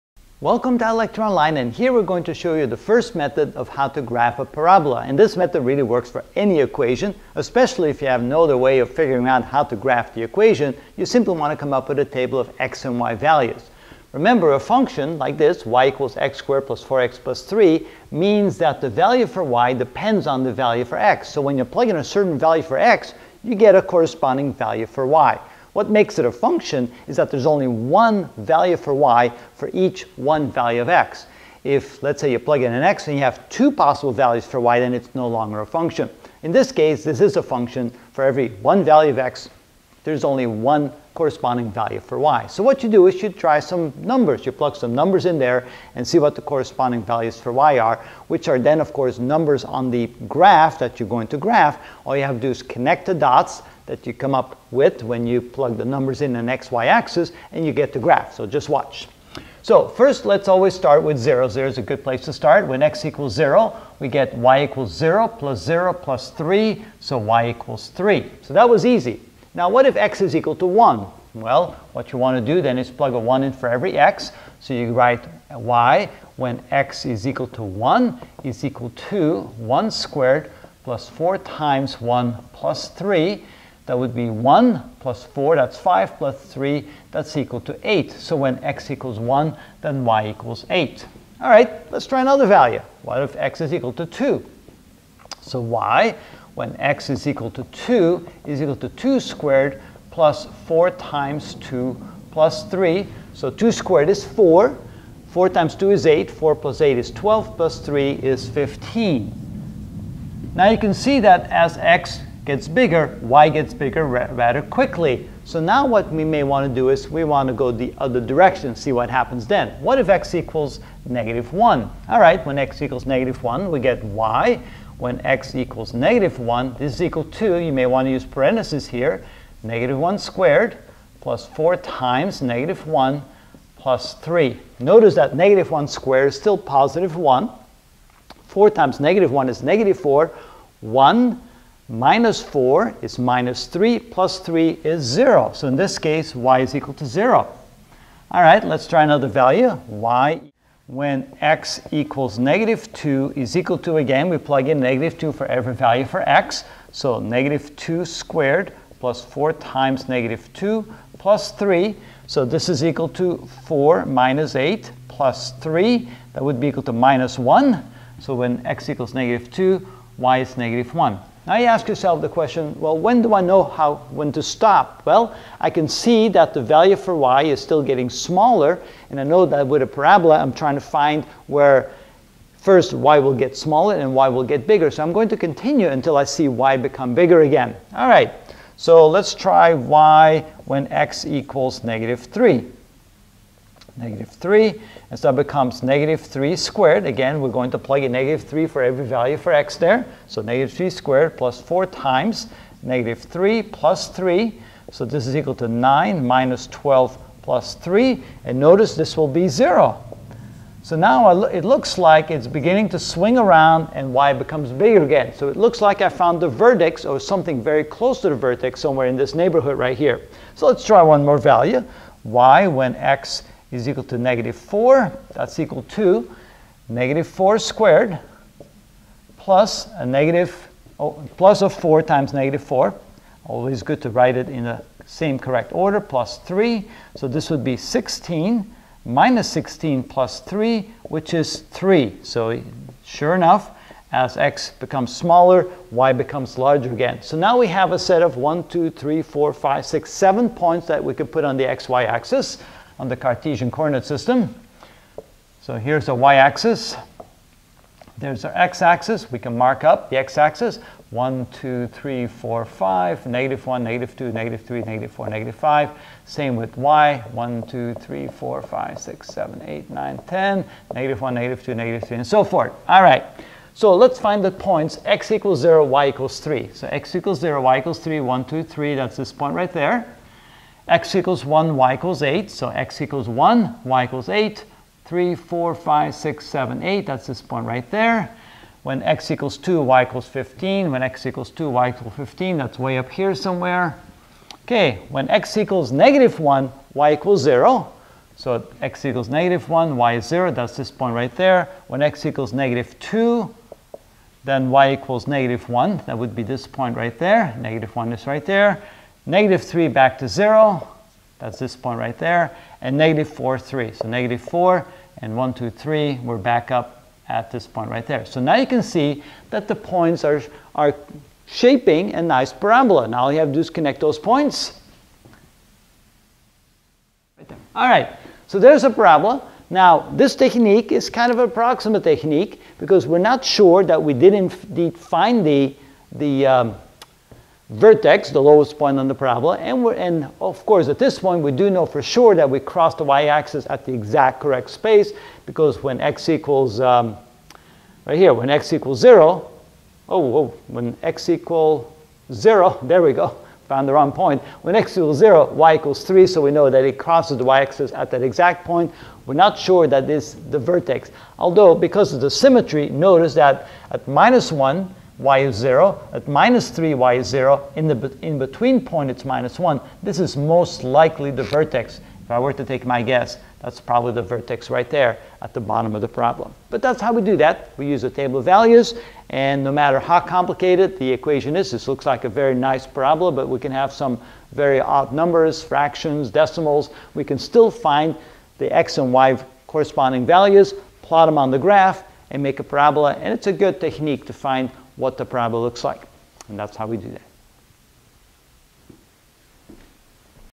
PreCalculus - Algebra Fundamental Review (67 of 80) 6 Ways to Graph a Parabola 1) Table of Values
In this video I will explain the table of x and y value method of graphing a parabola.